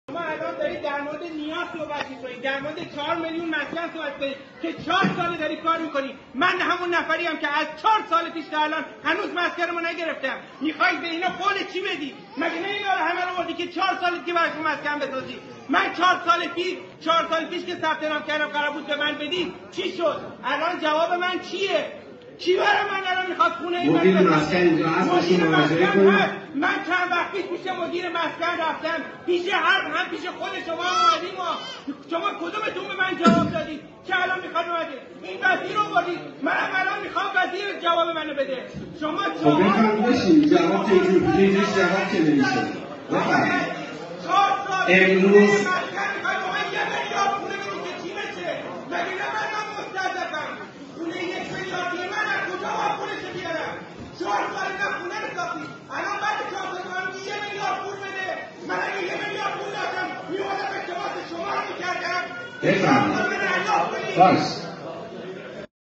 فریادهای اعتراضی یک شهروند اهوازی هنگام سخنرانی استاندار اهواز
فریادهای اعتراضی یک شهروند اهوازی هنگام سخنرانی استاندار اهواز در جلسه ای که با حضور بذرپاش وزیر راه و شهرسازی  حکومت تهران

این شهروند به وعده های مدیران استان و حکومت تهران در خصوص نهضت مسکن و عدم تحویل واحد مسکونی مربوطه علیرغم گذشت قریب به ۴ سال پیاپی معترض بود